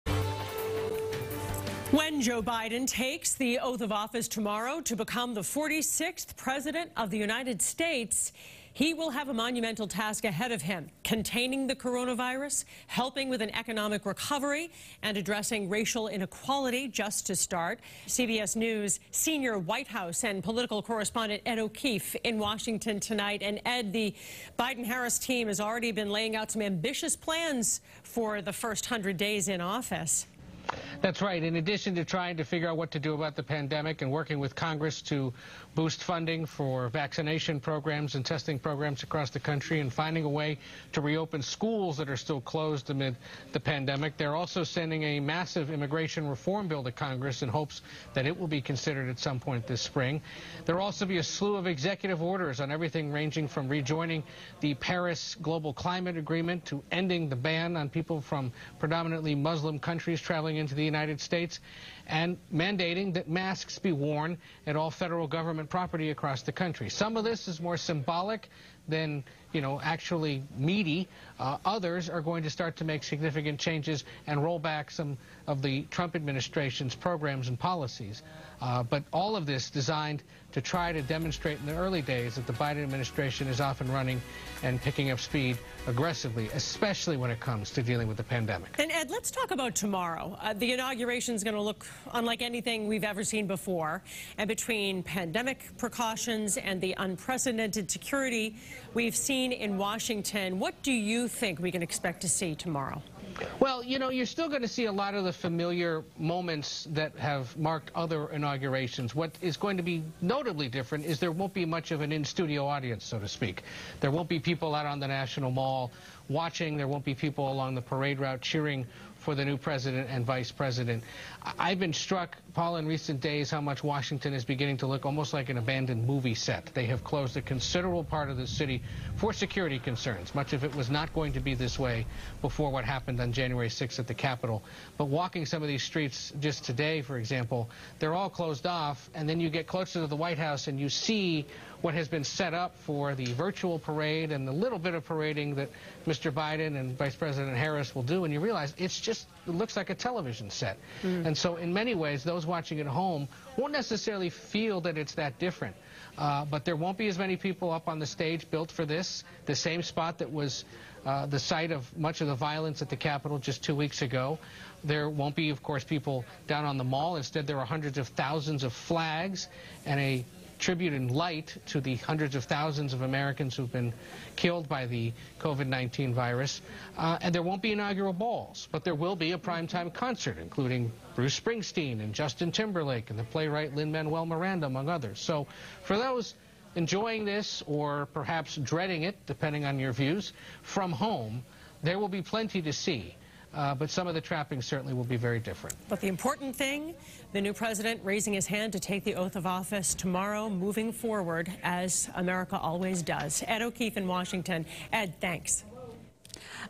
Ed O'Keefe On Inauguration Day, Biden's Ambitious Plans
CBS Senior White House Correspondent Ed O'Keefe discusses some of President-Elect Joe Biden's ambitious plans and security in place ahead of Inauguration Day.